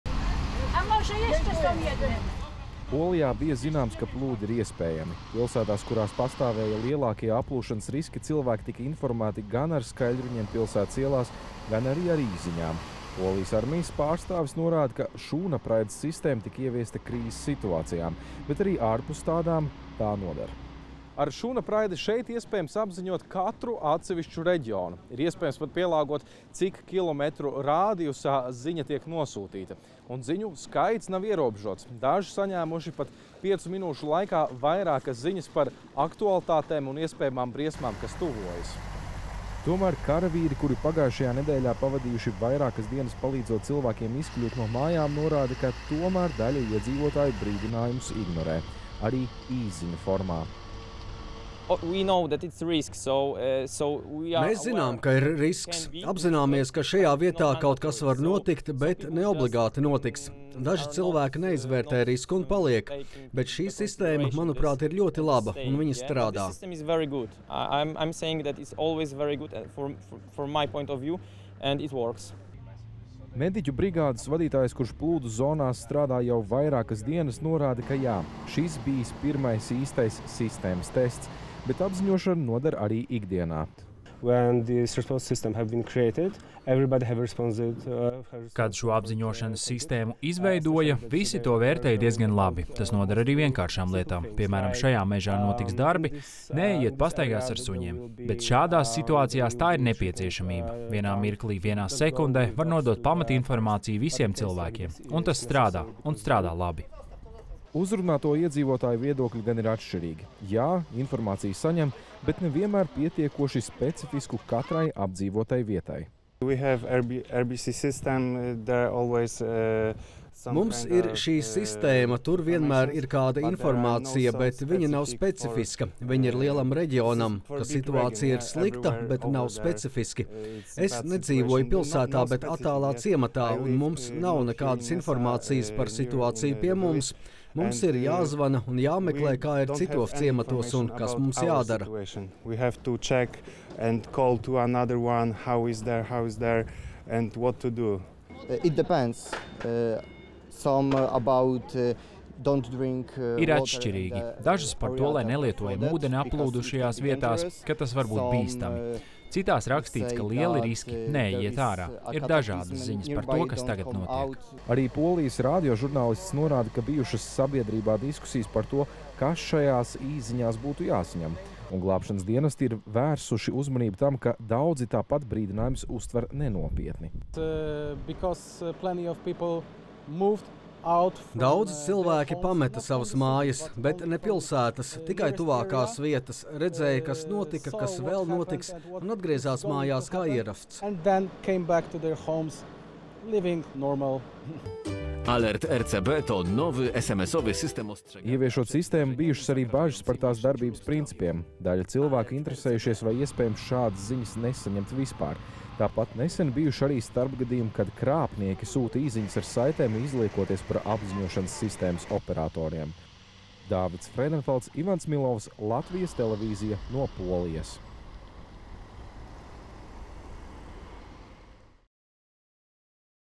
Polijas plūdi bija kā pārbaudījums iedzīvotāju apziņošanas sistēmai
Polijā darbojas iedzīvotāju apziņošanas sistēma, kas ļauj iedzīvotājiem saņemt īsziņas par aktuālāko savā reģionā un draudiem, kuri tuvojas. Plūdi Polijā bijuši kā kārtīgs pārbaudījums šai apziņošanas sistēmai. Latvijas Televīzijas uzrunāto cilvēku viedokļi gan atšķīrās. Vieni norāda, ka sistēma strādā lieliski, citi, ka ne visas saņemtās ziņas bijušas pamatotas.